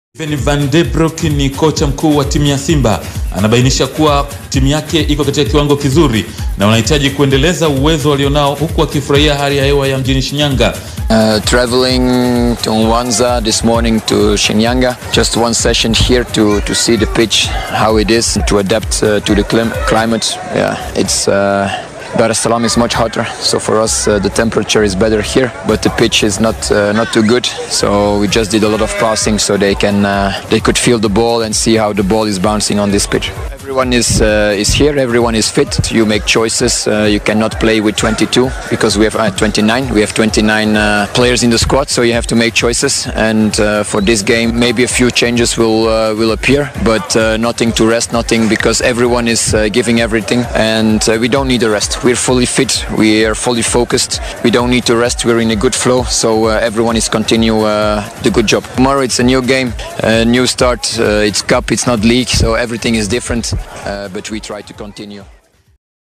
SIMBA WAAHIDI MAANGAMIZI, MABADILIKO MACHACHE YA KIKOSI HALI YA HEWA...
Simba Vs Stand# Pitch ya uwanja yamshtua kocha